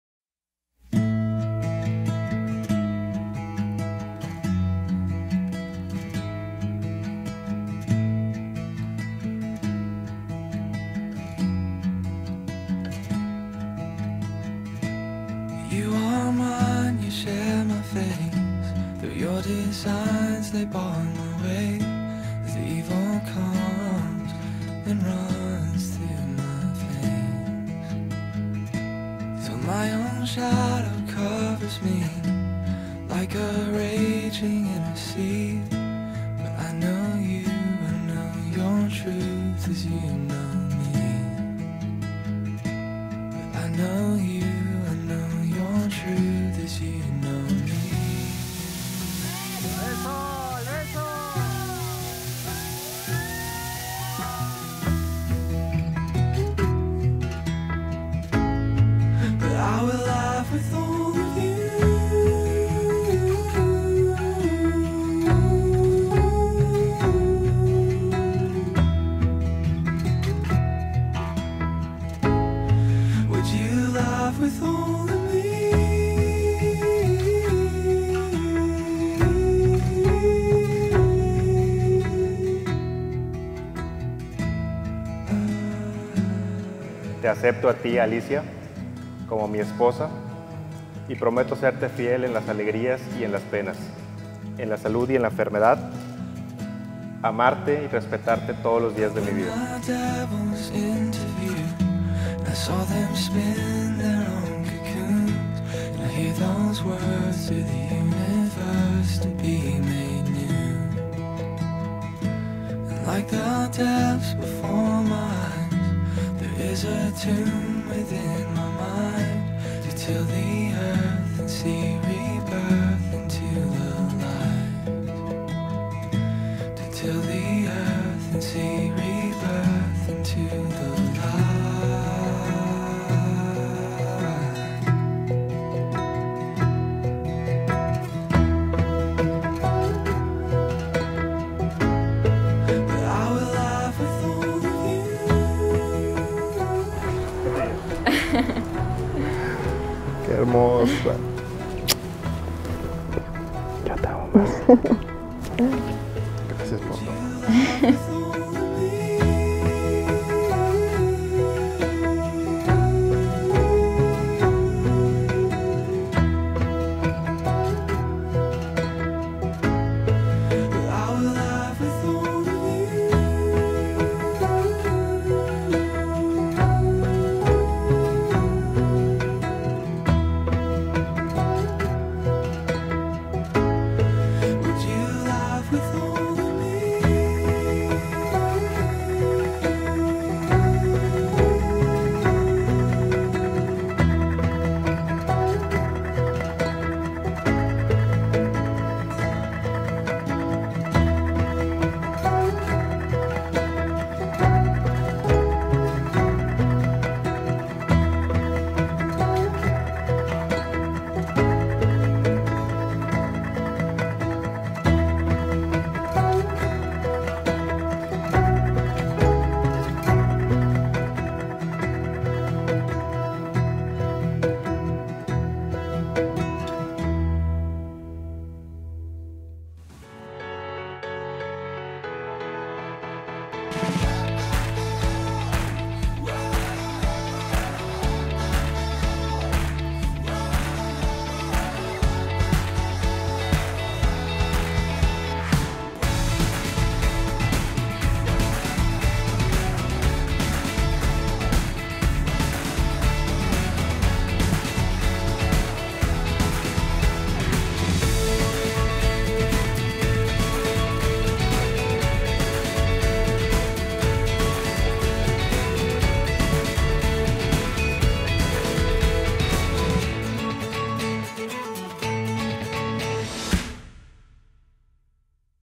Film Corto Alicia+Misael en La Cantera Eventos // Atelier 4/8 Wedding Photographers 4K
Boda realizada en la ciudad de Santiago, Nuevo Leon

Recepcion: La Cantera Eventos
Iglesia: Parroquia Santiago Apostol
Sesion de Fotos: Fabrica de Textiles El Porvenir en el Cercado, NL
Vestido: Adriana Madrid
Tocado: El taller